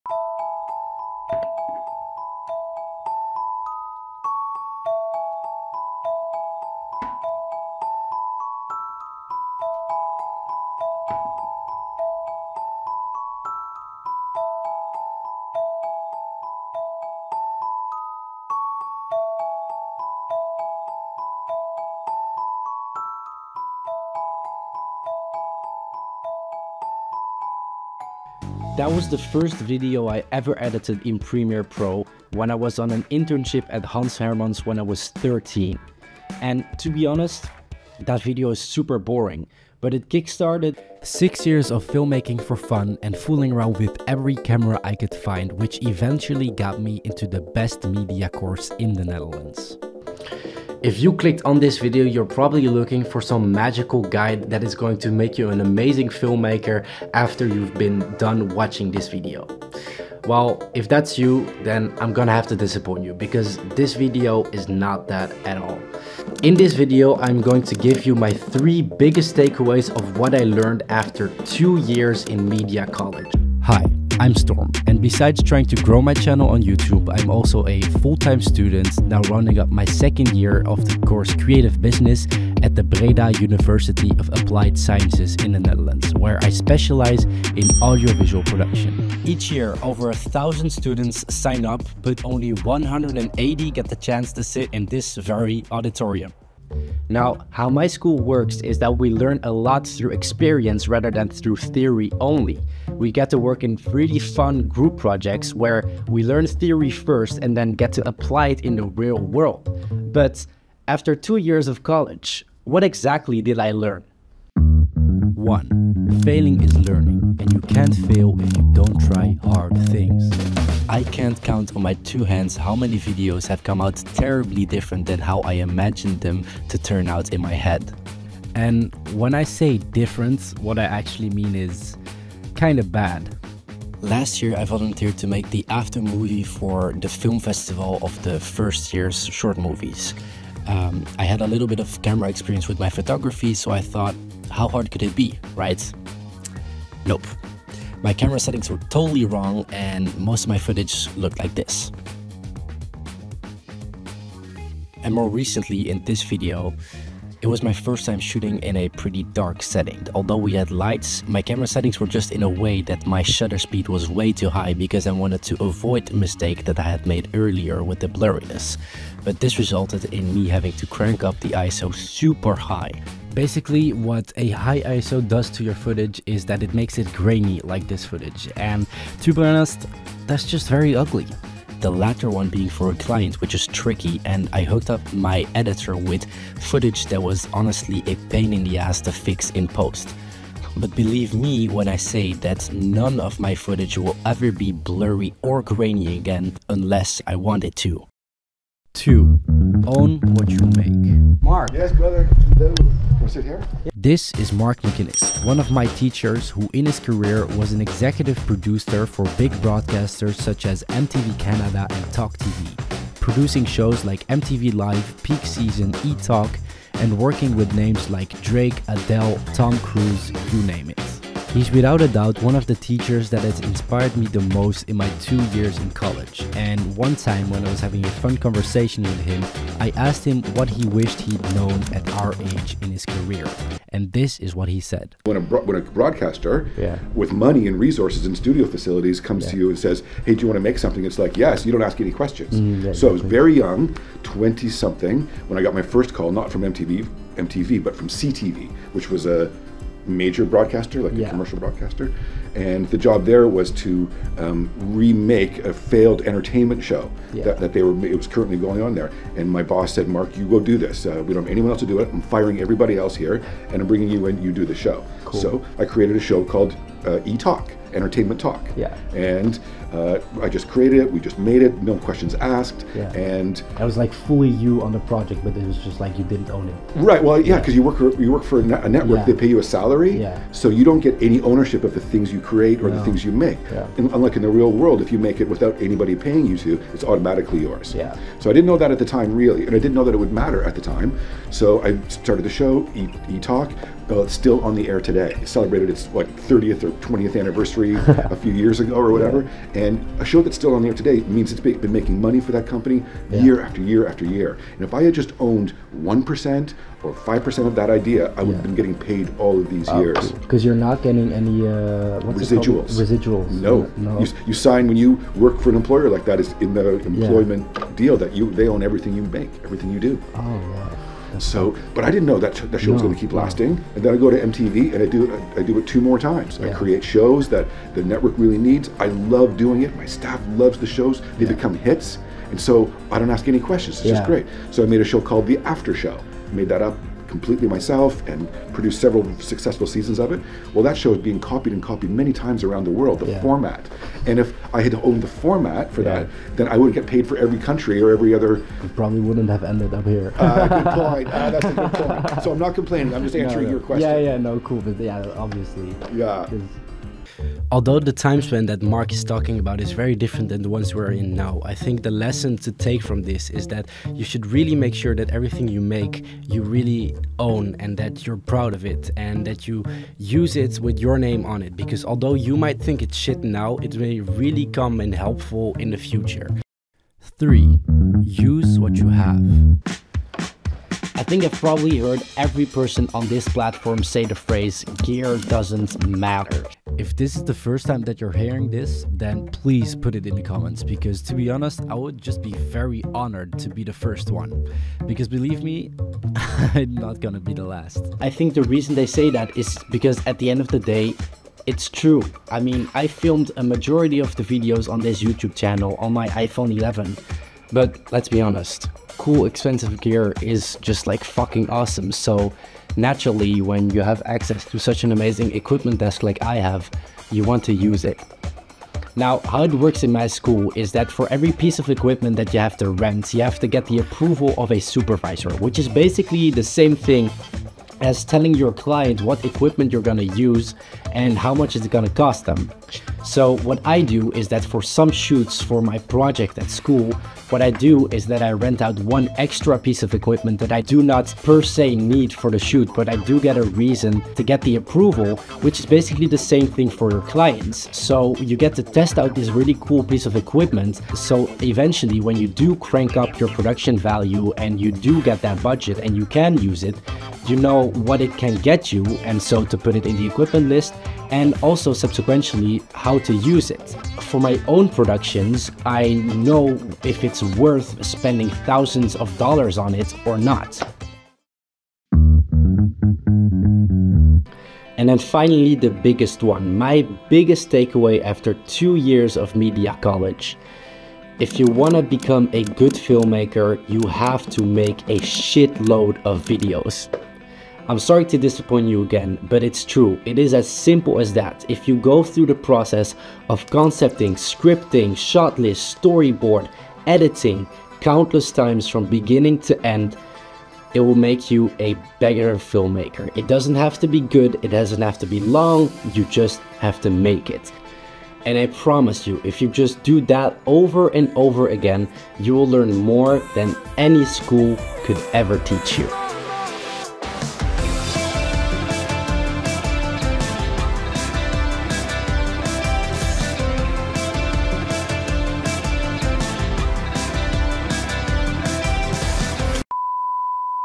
What I learned in 2 YEARS of Media college in UNDER 10 MINUTES
This video was made in the style of @DanMace as part of my series where I copy the styles of my favourite YouTubers, dan Is really an amazing filmmaker so check out his stuff!